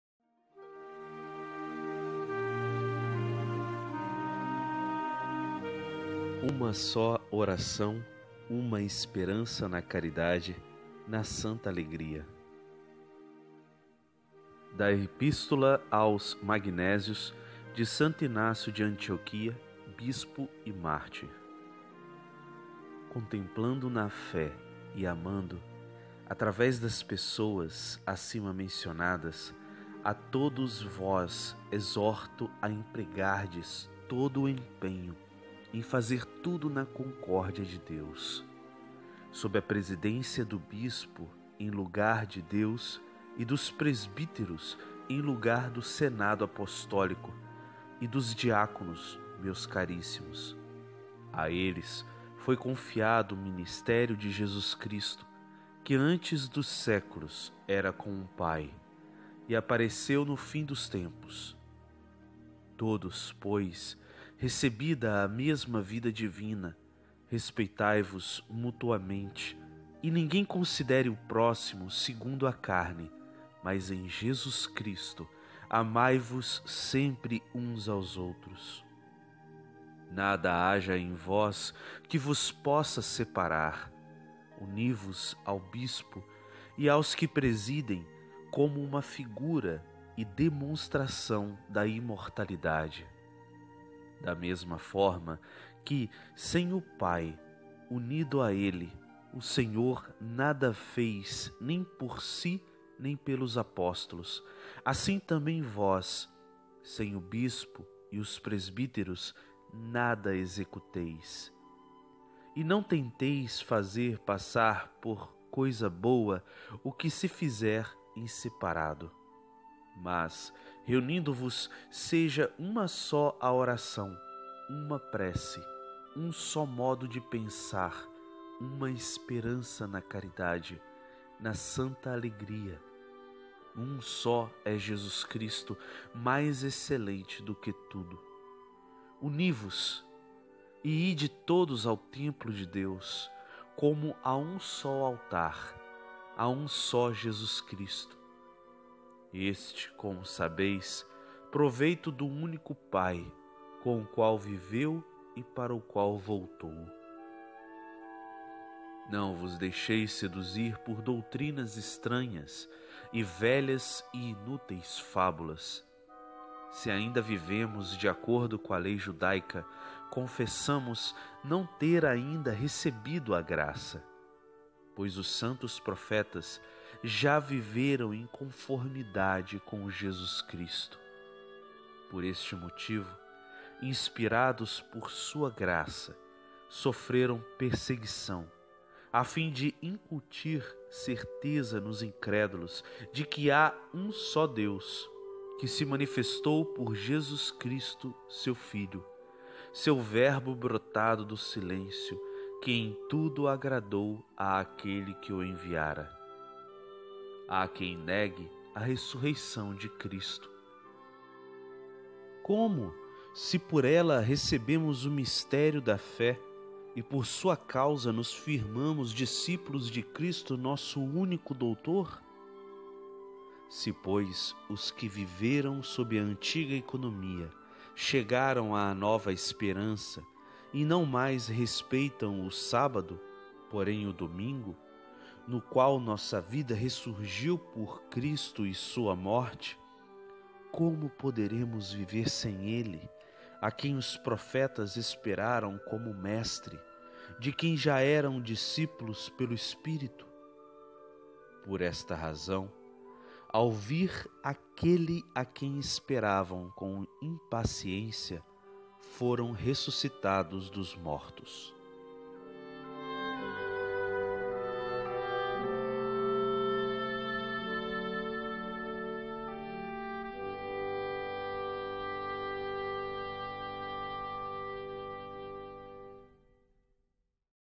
335-Uma só oração, uma esperança na caridade, na santa alegria
🗓Segunda-feira da 16ª Semana do Tempo Comum

Da Epístola aos magnésios, de Santo Inácio de Antioquia, bispo e mártir.

Canal no Telegram com 'áudios-leitura' de textos da Patrística  retirado do Ofício das Leituras da Liturgia das Horas.